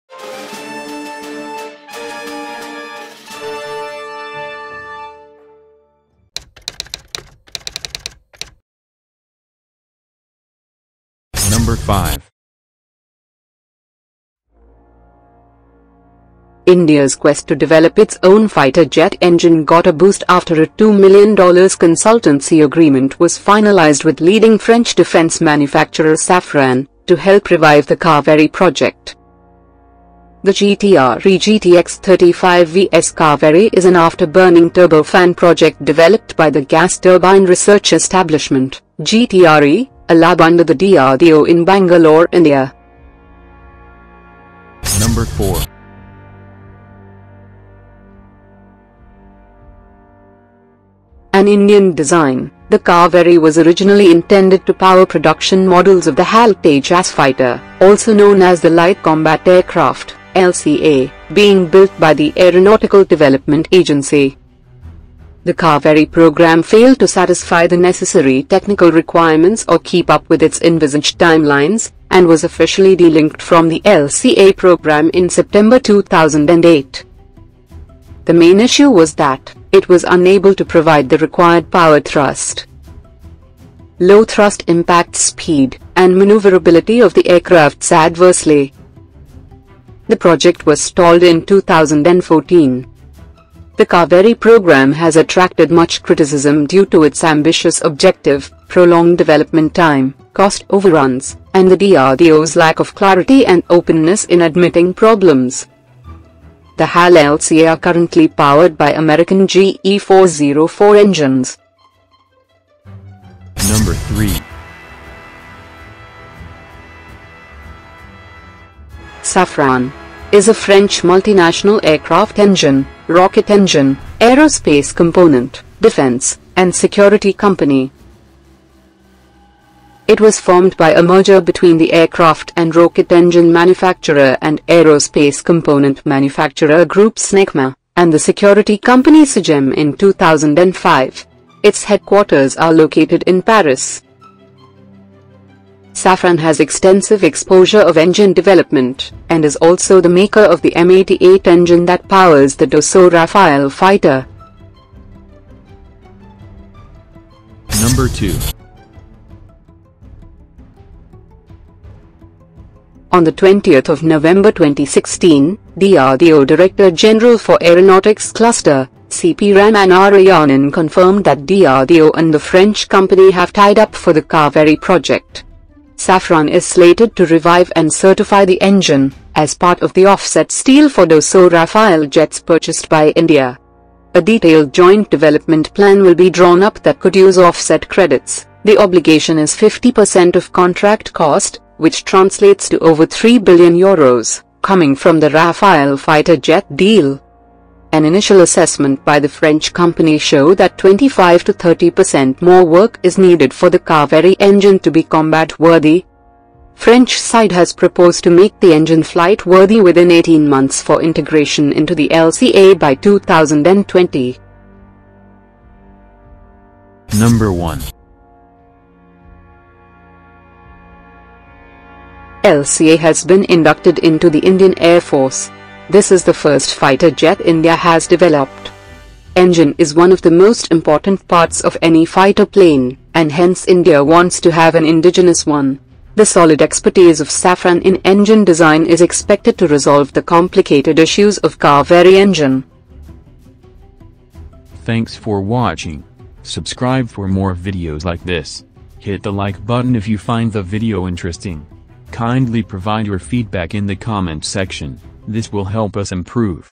INDIGENOUS KAVERI ENGINE PROJECT TO BE REVIVED WITH FRENCH HELP: TOP 5 FACTS
INTRODUCTION:

India’s quest to develop its own fighter jet engine got a boost after a $2-million consultancy agreement was finalized with leading French defense manufacturer Safran to help revive the Kaveri project.

The GTRE GTX-35VS Kaveri is an after burning turbofan project developed by the Gas Turbine Research Establishment (GTRE), a lab under the DRDO in Bangalore India.

BACKGROUND:

An Indian design, the Kaveri was originally intended to power production models of the HAL Tejas fighter, also known as the Light Combat Aircraft (LCA) being built by the Aeronautical Development Agency.

The Kaveri program failed to satisfy the necessary technical requirements or keep up with its envisaged timelines, and was officially delinked from the LCA program in September 2008. The main issue was that, it was unable to provide the required power thrust.

Low thrust affects speed and maneuverability of the aircrafts adversely.

The project was stalled in 2014.

The Kaveri program has attracted much criticism due to its ambitious objective, prolonged development time, cost overruns, and the DRDO's lack of clarity and openness in admitting problems.

The Hal LCA are currently powered by American GE 4 0 4 engines.

SAFRAN:

Safran. is a French multinational aircraft engine, rocket engine, aerospace-component, defense, and security company. It was formed by a merger between the aircraft and rocket engine manufacturer and aerospace component manufacturer group SNECMA, and the security company SAGEM in 2005. Its headquarters are located in Paris.

Safran has extensive exposure of engine development, and is also the maker of the M88 engine that powers the Dassault Rafael fighter.

CURRENT STATE:

On 20 November 2016, DRDO Director General for Aeronautics Cluster, C P Ramanarayanan confirmed that DRDO and the French company have tied up for the Kaveri project.

Safran is slated to revive and certify the engine, as part of the offsets deal for Dassault Rafael jets purchased by India.

A detailed joint development plan will be drawn up that could use offset credits — the obligation is 50% of contract cost, which translates to over 3 billion euros — coming from the Rafael fighter jet deal.

An initial assessment by the French company show that 25-30% more work is needed for the Kaveri engine to be combat-worthy.  French side has proposed to make the engine flight-worthy within 18 months for integration into the LCA by 2020.

CONCLUSION:

LCA has been inducted into the Indian Air Force. This is the first fighter jet India has developed.

Engine is one of the most important parts of any fighter plane, and hence India wants to have an indigenous one.

The solid expertise of Safran in engine design is expected to resolve the complicated issues of Kaveri engine.